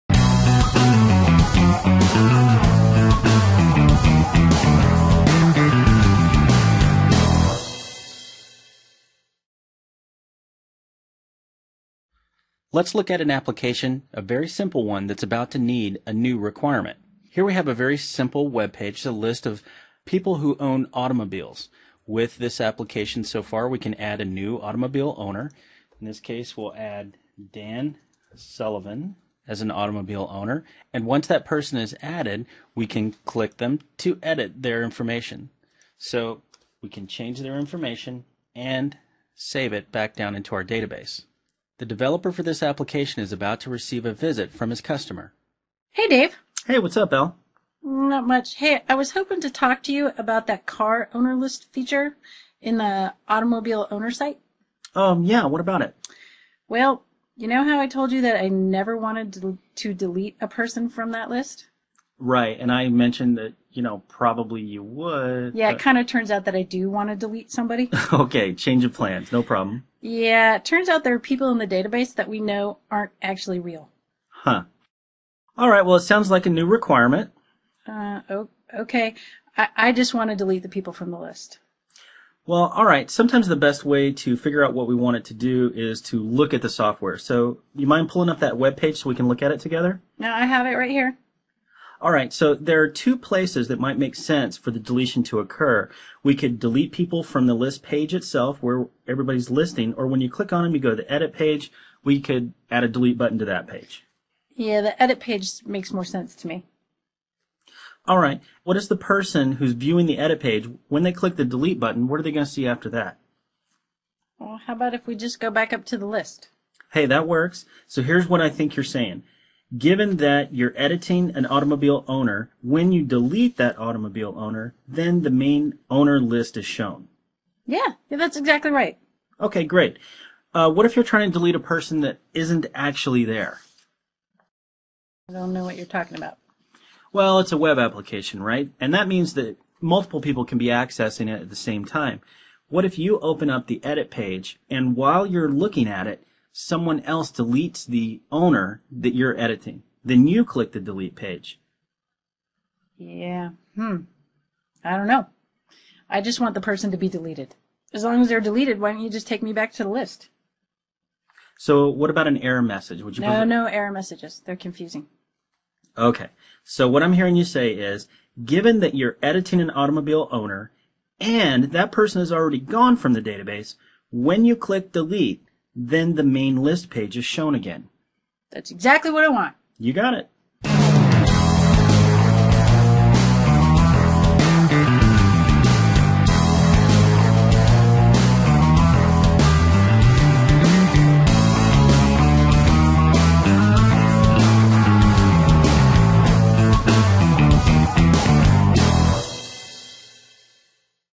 Test First Development Part 2 | Pluralsight
Learn how to talk to women (and other users)!  In this video excerpt from David Starr's "Test First Development Part 2" shows how turn a conversation with his user, sultry auburn haired cartoon Lisa, into the concept of a "Desirement" as expressed in a user story and requirements expressed as scenarios.  These are key factors in Test First Development.

Test First Development | Pluralsight